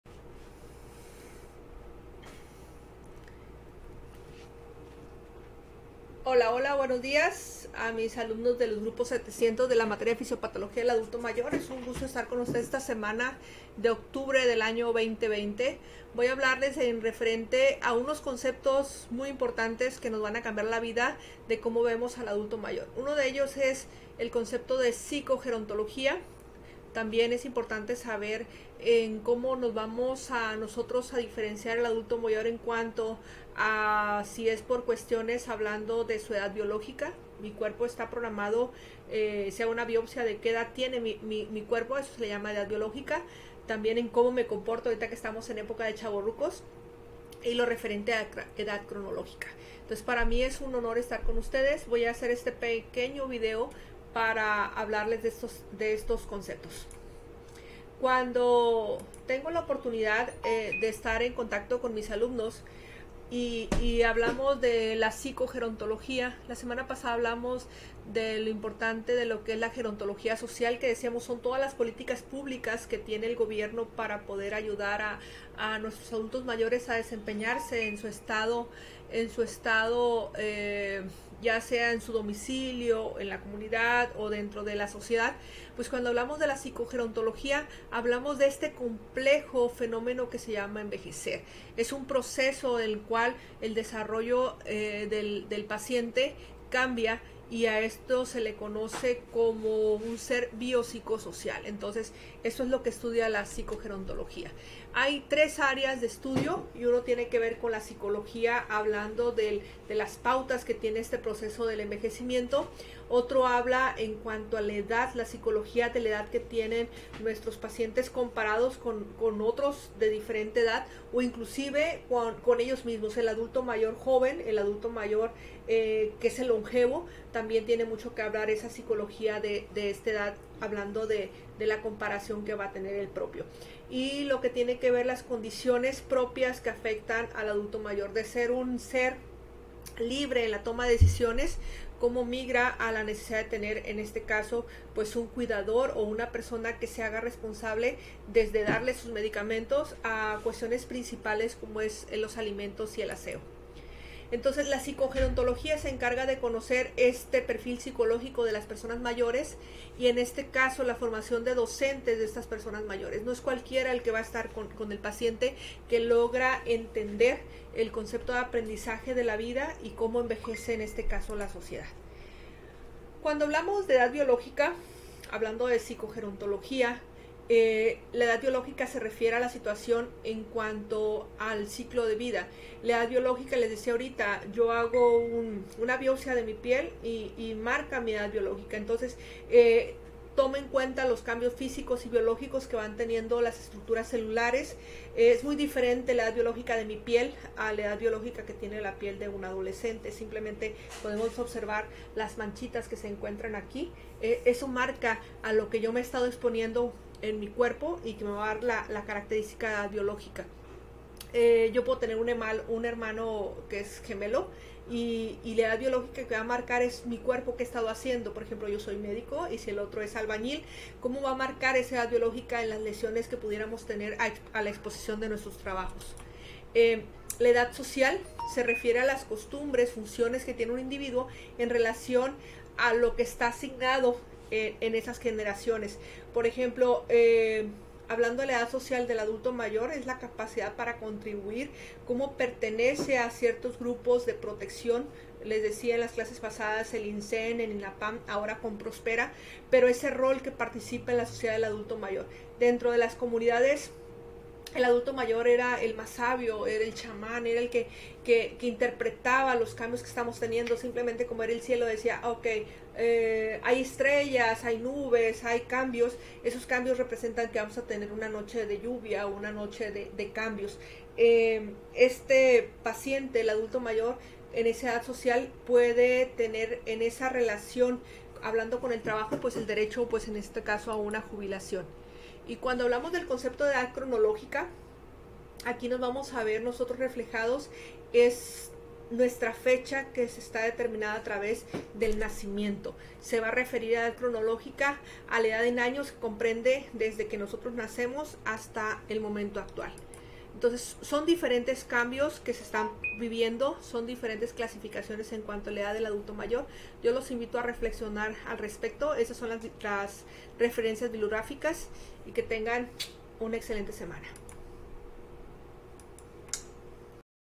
Psicogerontología; Edad social, cronológica y biológica
En este video se analiza los conceptos de Psicogerontología, Edad Biológica, social y Cronológica.